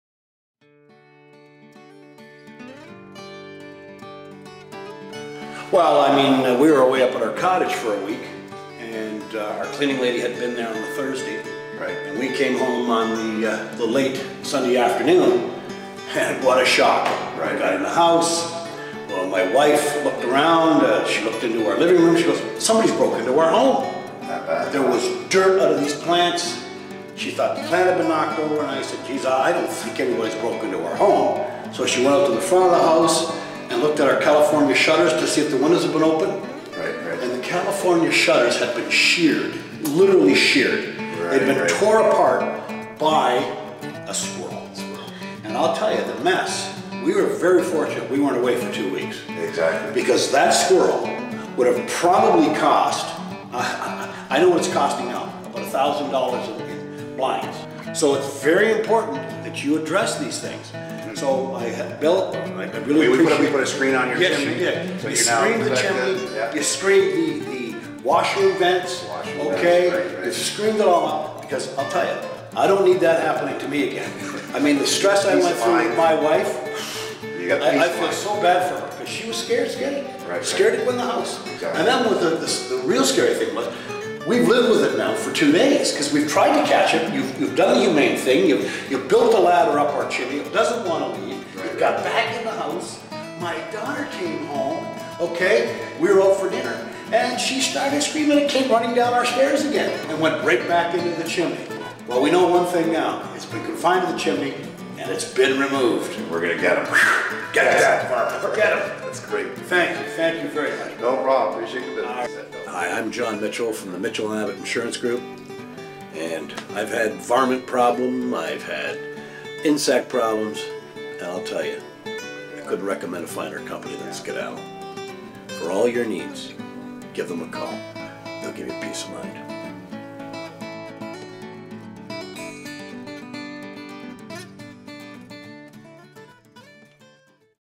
John Mitchell Thank You
John Mitchell tells the story of how he discovered his messy squirrel problem. From dirt tossed out of indoor plants to shredded California shutters, John’s home had some serious squirrel damage. Luckily he called Skedaddle! Our technicians safely removed the squirrel and screened his chimney and washing vent covers so they would not be able to return.